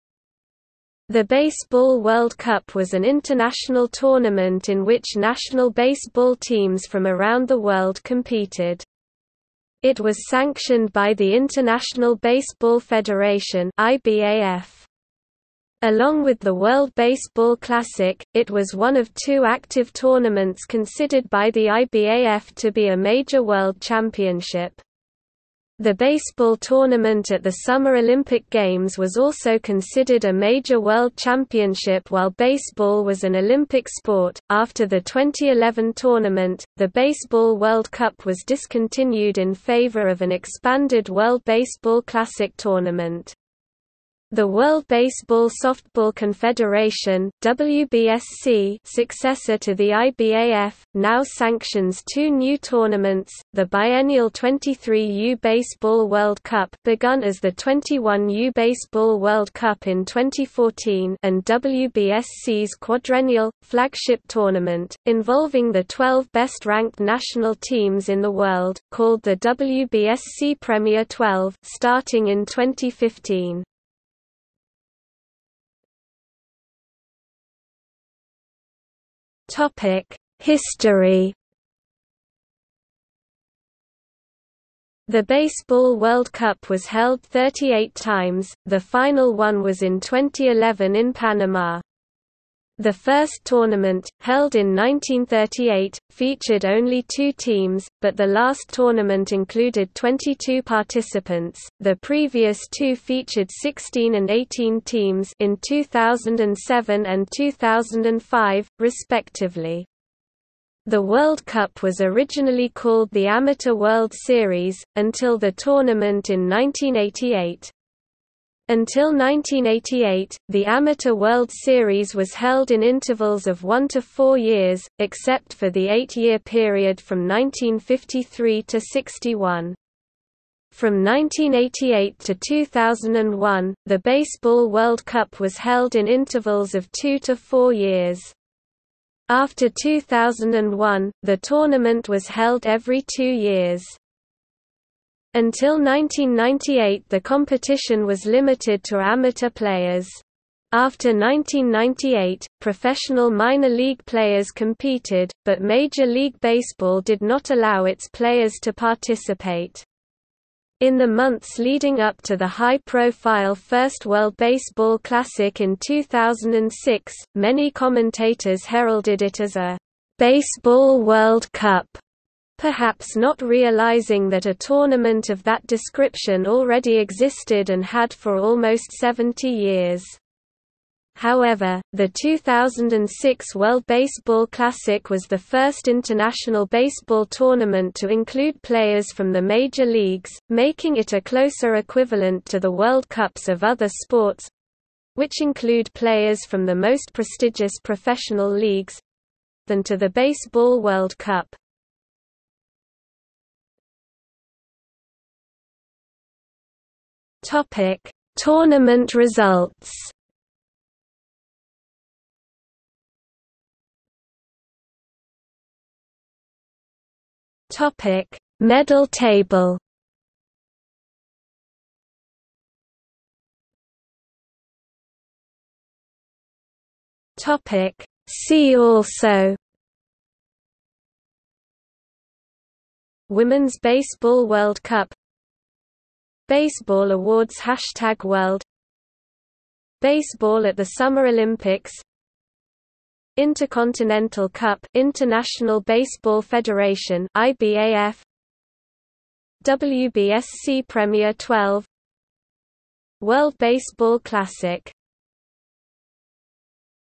World Cup of Baseball | Wikipedia audio article
00:01:15 1 History
00:03:23 2 Tournament results
00:03:34 3 Medal table
00:03:43 4 See also

- Socrates

SUMMARY
The Baseball World Cup was an international tournament in which national baseball teams from around the world competed. It was sanctioned by the International Baseball Federation (IBAF). Along with the World Baseball Classic, it was one of two active tournaments considered by the IBAF to be a major world championship. The baseball tournament at the Summer Olympic Games was also considered a major world championship while baseball was an Olympic sport.After the 2011 tournament, the Baseball World Cup was discontinued in favor of an expanded World Baseball Classic tournament. The World Baseball Softball Confederation (WBSC) – successor to the IBAF – now sanctions two new tournaments: the biennial 23U Baseball World Cup (begun as the 21U Baseball World Cup in 2014) and WBSC's quadrennial, flagship tournament – involving the twelve best-ranked national teams in the world – called the WBSC Premier12 (starting in 2015).